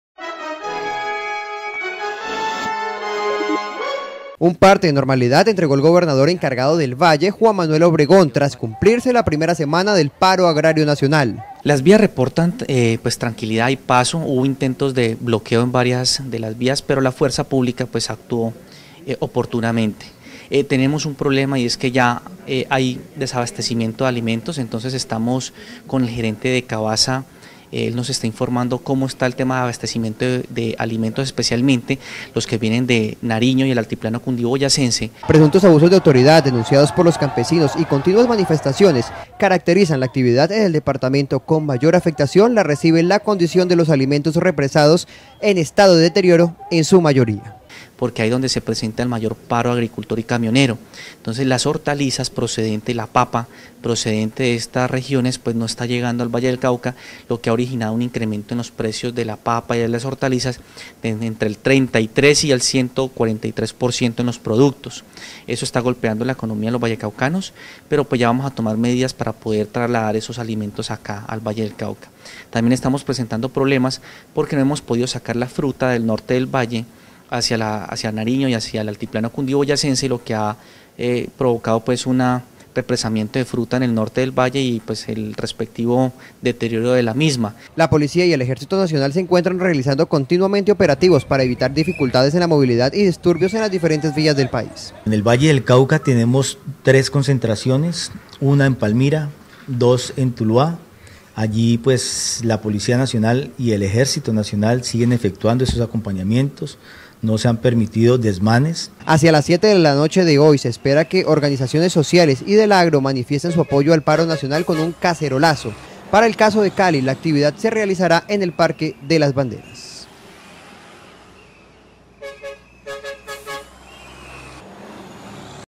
Las vías del Valle no están afectadas por el paro nacional
Un parte de normalidad entrego el Gobernador encargado del Valle Juan Manuel Obregón, tras cumplirse la primera semana del paro agrario nacional.

Presuntos abusos de autoridad denunciados por los campesinos y continuas manifestaciones, caracterizan la actividad en el departamento con mayor afectación la recibe la condición de los alimentos represados en estado de deterioro en su mayoría.

La Policía y el ejército  nacional se encuentran realizando continuamente operativos para evitar dificultades en la movilidad y disturbios en las diferentes vías del país.

Hacía las 7 de la noche de hoy se espera que organizaciones  sociales y del agro  manifiesten  su apoyo al paro nacional con un "Cacerolazo", para el caso de Cali, la actividad se realizará en el parque de las banderas.